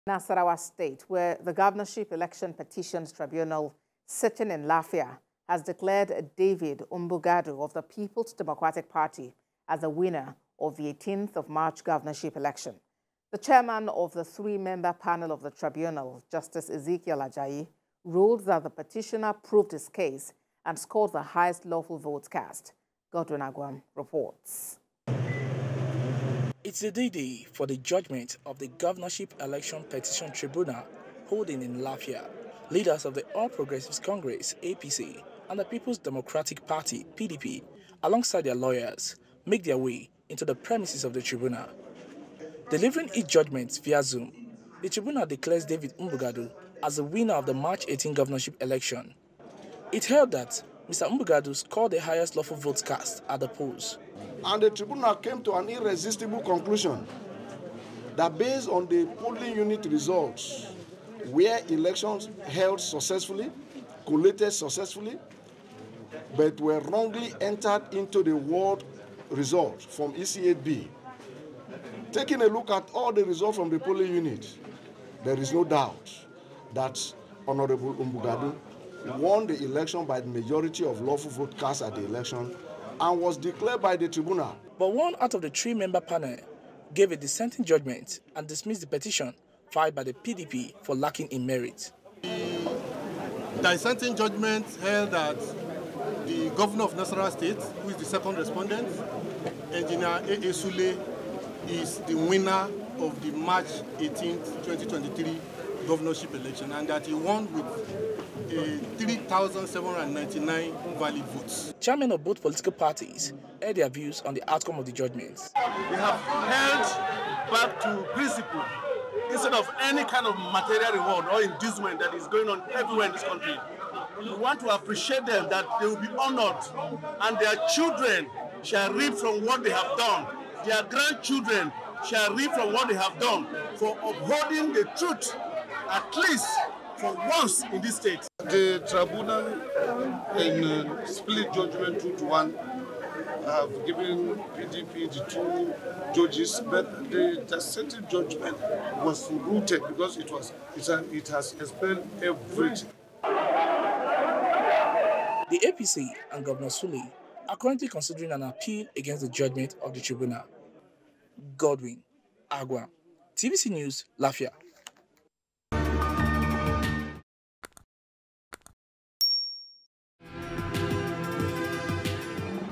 Tribunal Removes Governor Sule, Declares PDP Candidate Winner In Nasarawa State
In Nasarawa State the Governorship Election Petition Tribunal sitting in Lafia has declared David Umbugadu of the People's Democratic Party as the winner of the March 18 governorship election.
The Chairman of the three-man panel of the Tribunal, Justice Ezekiel Ajayi, ruled that the petitioner proved his case and scored the highest lawful votes cast.